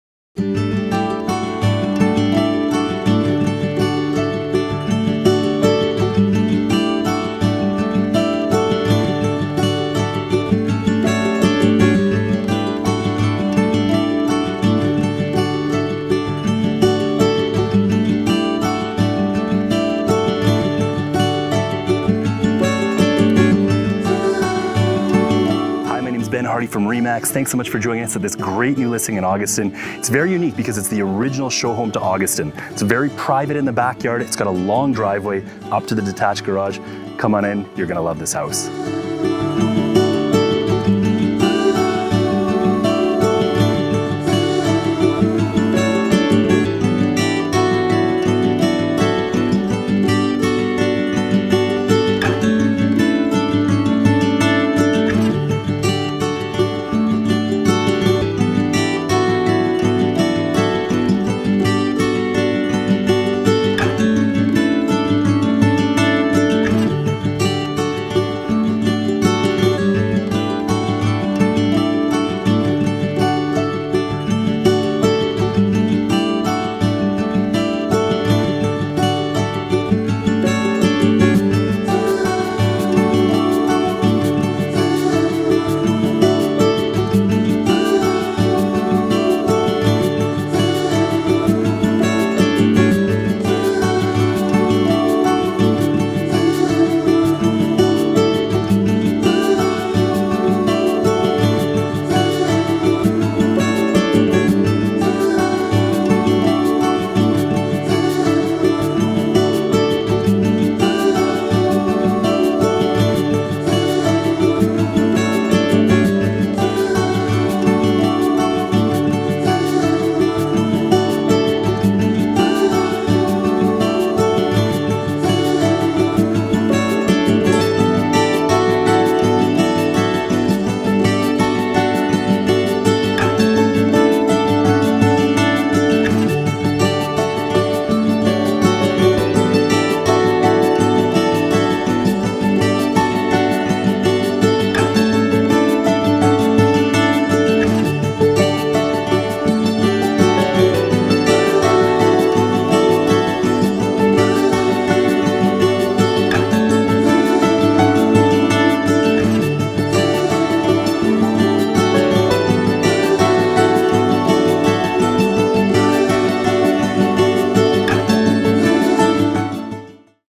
Original Auguston Show Home on Unique Lot with Great Views in Abbotsford
Welcome to 36310 Auguston Parkway S in Abbotsford, BC. Your new home search stops here! Step into this unique two-storey w/bsmt home & you will appreciate all that this original Auguston Show Home had to offer. Unique lot with its great views & rear lane access & a long driveway leading up to your lg, detached double garage. You will love the amazing privacy with the mature landscaping! With 4 beds & laundry upstairs, this is the perfect family home! You will love the spacious kitchen, crown mouldings, hardwood floors & the 9ft ceiling on the main. Send the kids downstairs to burn some energy in your lg rec room or play at the nearby parks. Other features include central A/C, a central vacuum system & a wonderful maintenance-free yard. Don't miss your chance to own a fantastic home in an amazing location; book your personal viewing today!